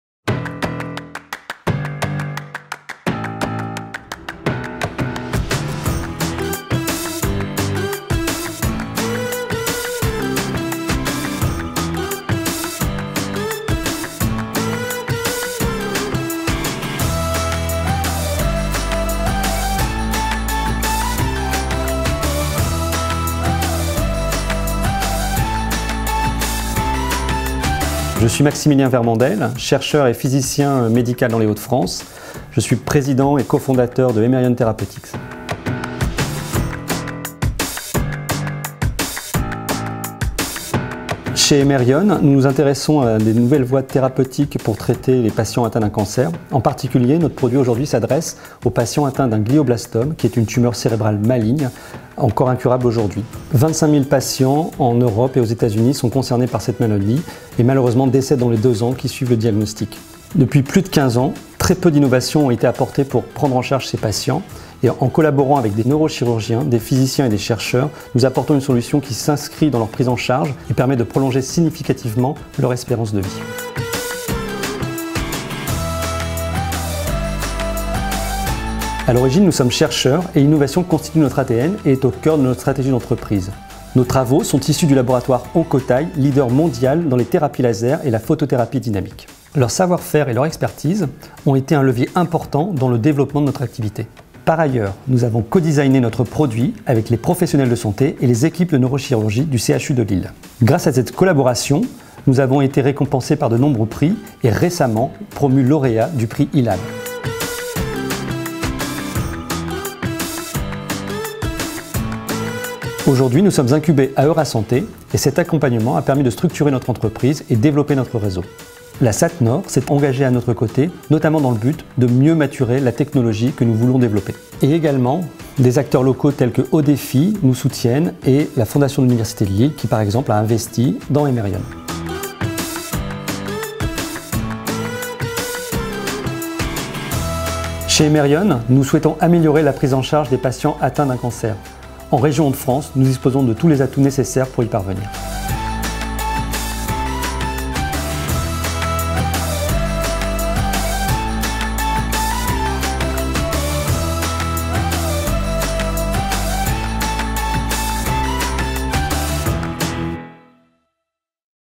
[Trophées de l'innovation] Le lauréat de la catégorie innovation thérapeutique est...
La remise des Trophées de l'innovation Hauts-de-France marque le lancement de cette nouvelle édition des Rencontres de la Recherche et de l'Innovation. À cette occasion, 5 innovations régionales sont récompensées pour leurs impacts positifs sur l'homme, l'environnement et la société. Découvrez Hemerion, primée dans la catégorie innovation thérapeutique.